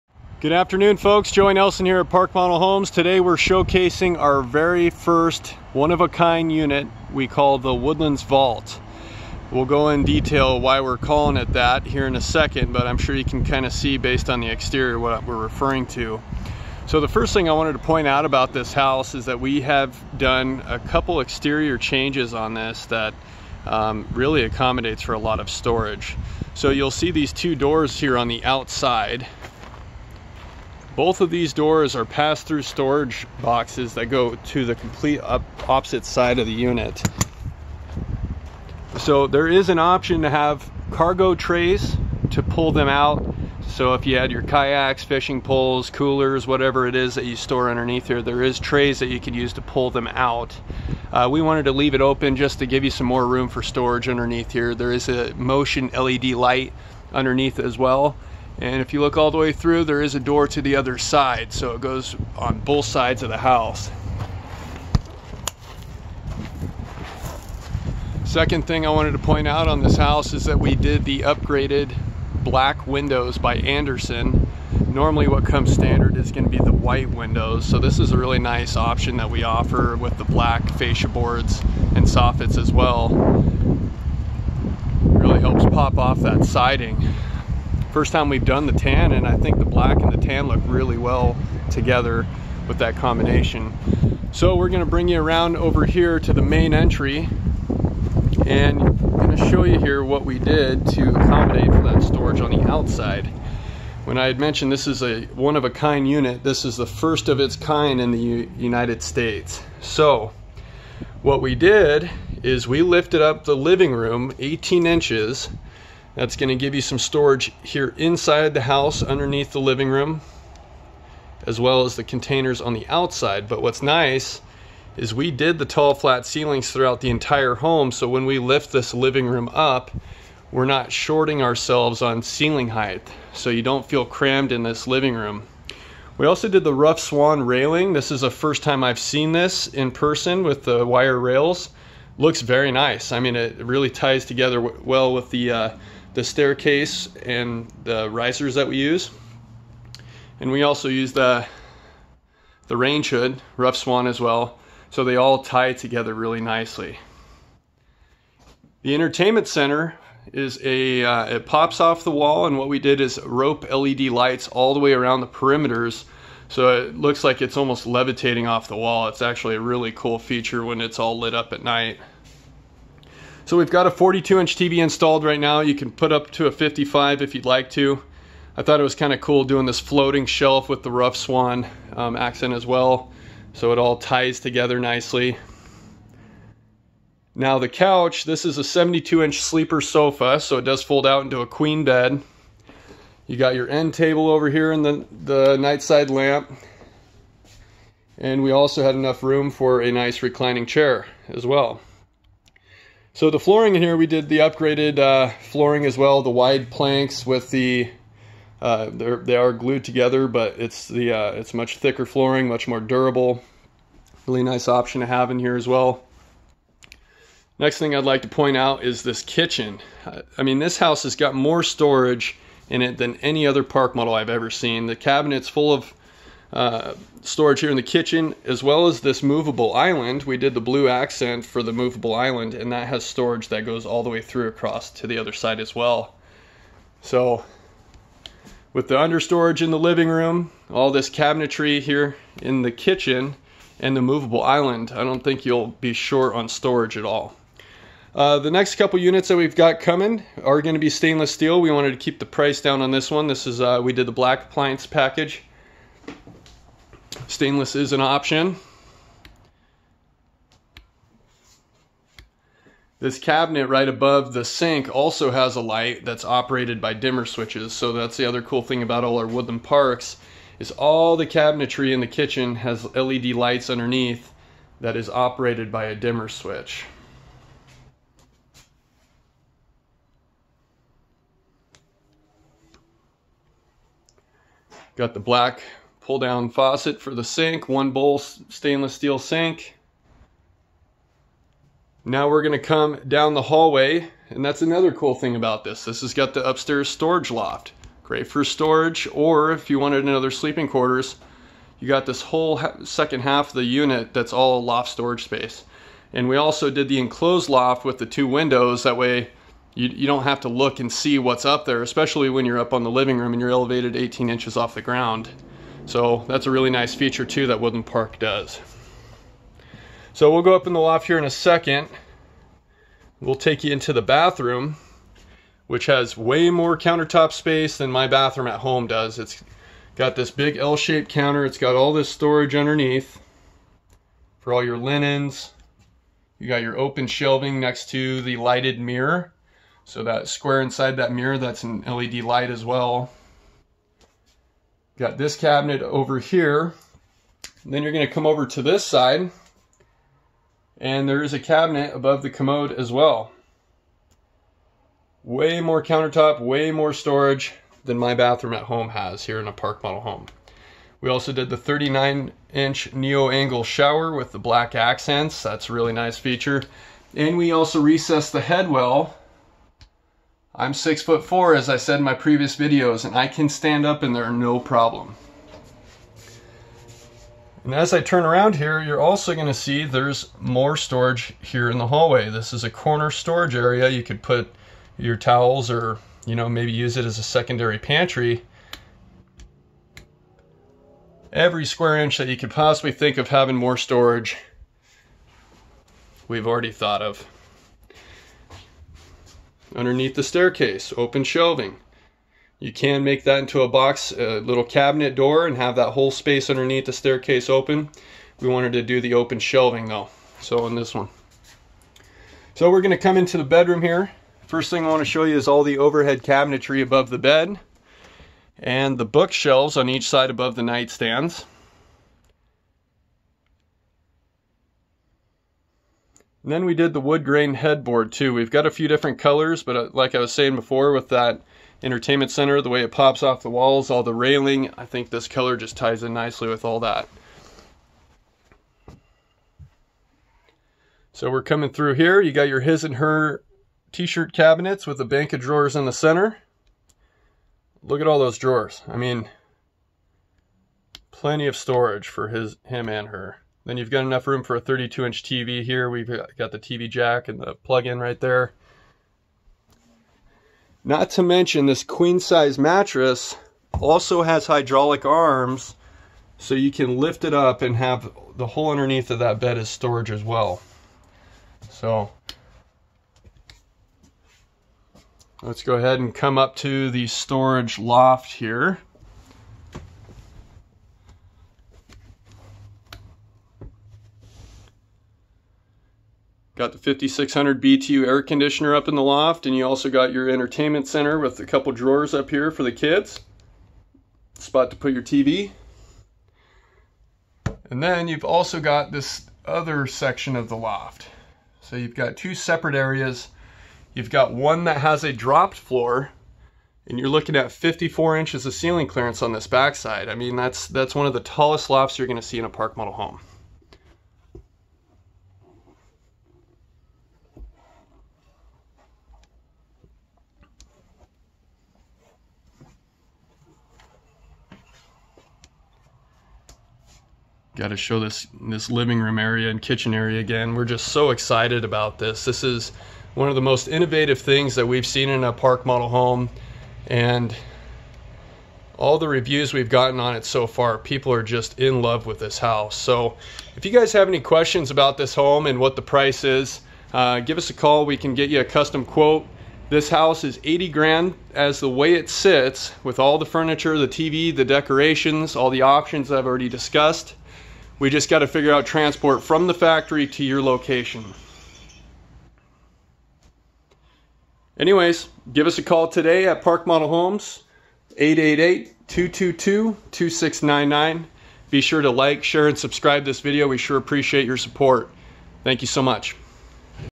LUXURY ONE-OF-A-KIND TINY HOME! YOU HAVE TO SEE THIS!
Looking for extra storage space in a roomy tiny house? The Woodlands Vault is a model that includes a special feature: a raised floor in the living room that offers a huge amount of extra storage underneath. Access to this special vault is available inside from the kitchen as well as through convenient pull-out compartments outside. Additional features include a home theater with room for a projector screen, a corner storage area in the hallway, open shelving under the stairs, an enclosed loft with two windows, and a comfortable downstairs bedroom. If you have any questions on getting a custom quote on your very own Park Model Tiny Home please call us directly at .